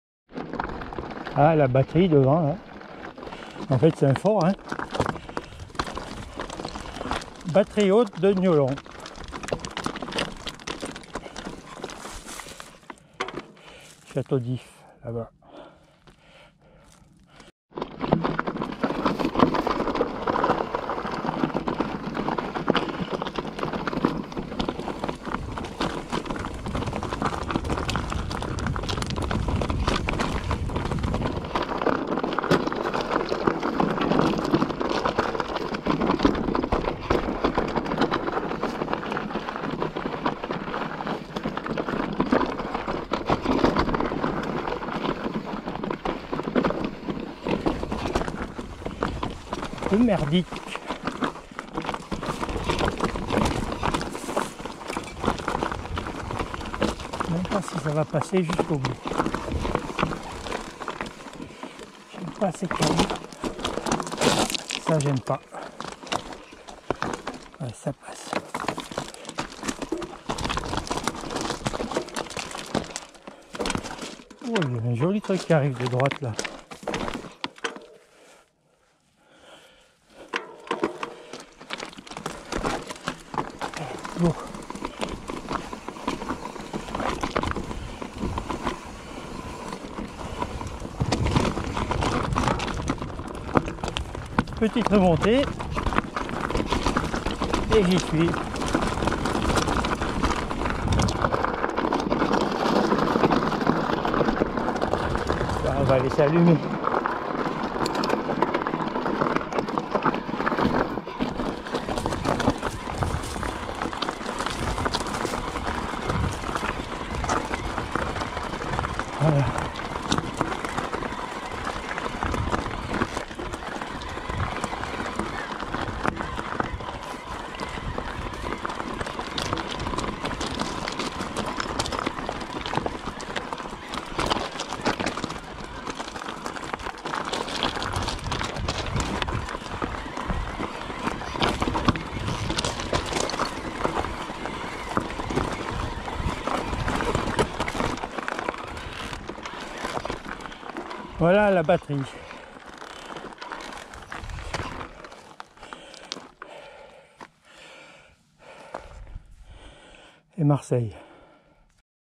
batterie de niolon 2020 2
test d'un vae du côté de niolon et de la batterie haute (13)